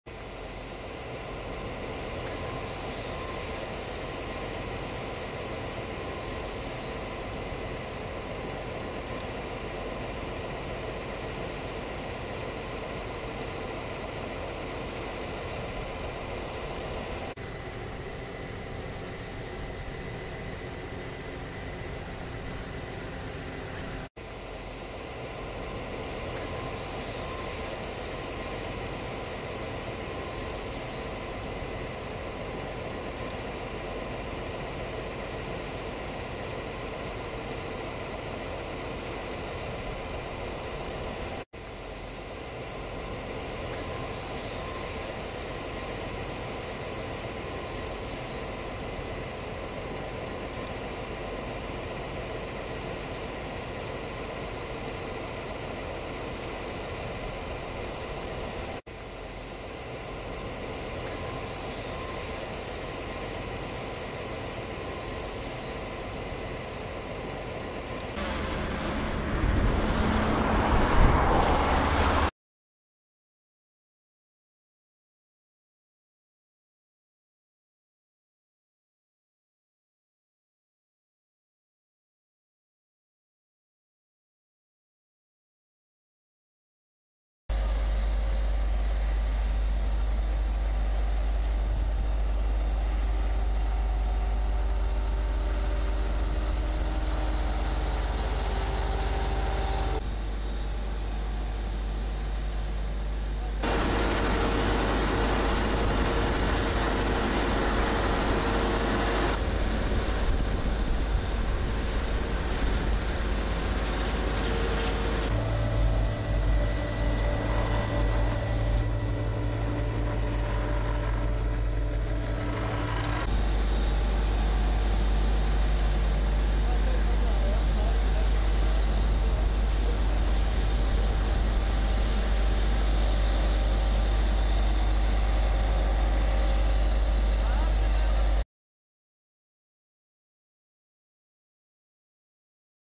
Hatay’da yol diye bir sorun konuşulmayacak
Asfaltlama çalışmaları dört koldan devam